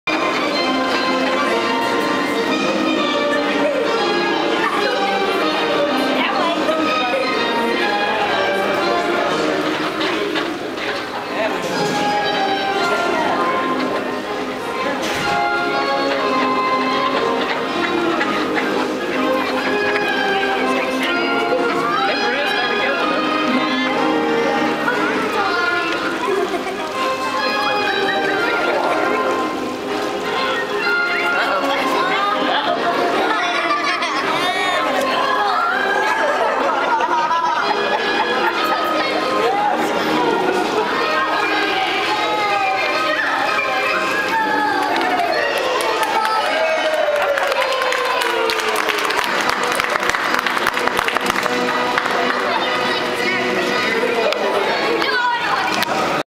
The Duck Walk
The ducks going off to work at the Peabody Hotel in Little Rock, AR